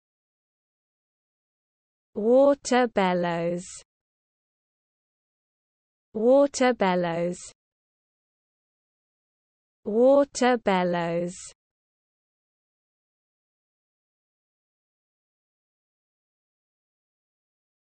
How to pronounce Water Bellows (CORRECTLY)
Learn how to correctly pronounce the word Water Bellows  in the US English accent.